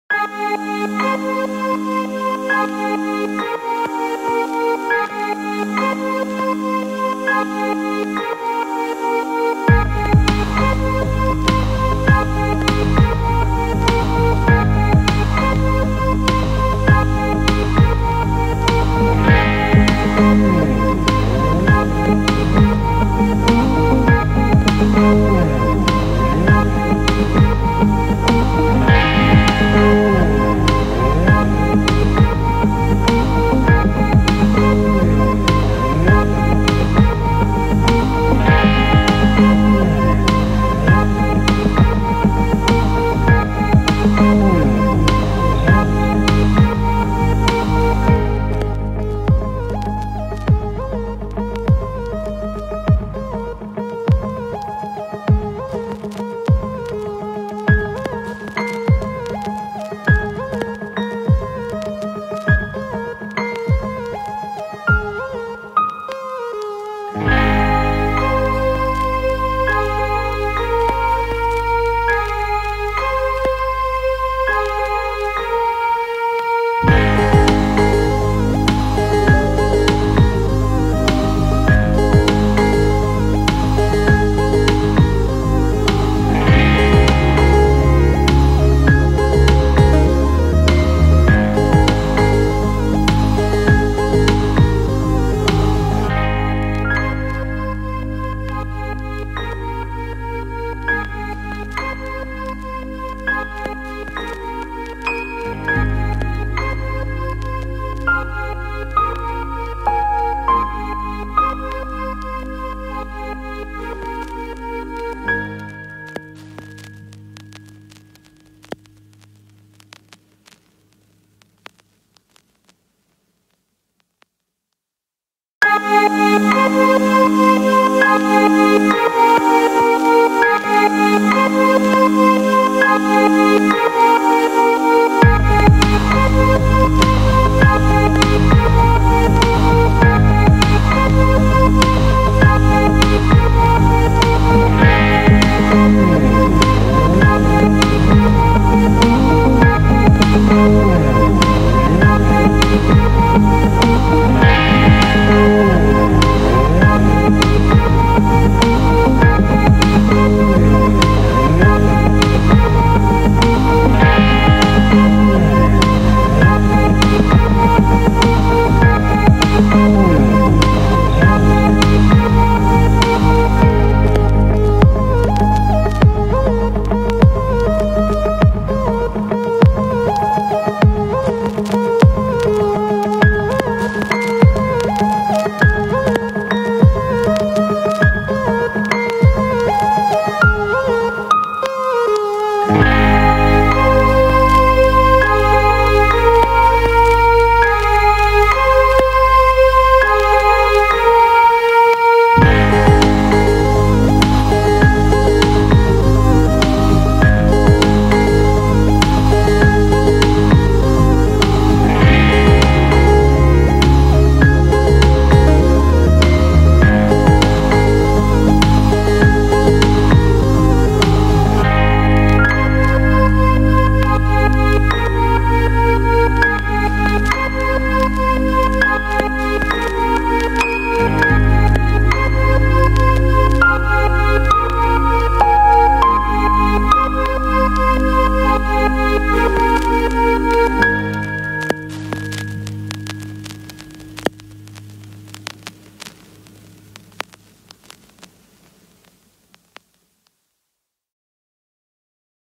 MYSQL.connector programmingError : character set 'Utf8' - python error fixed 2022
About the Video :
i have solve an error occurring during python update for while connect to the mysql, watch the video for solve such as that error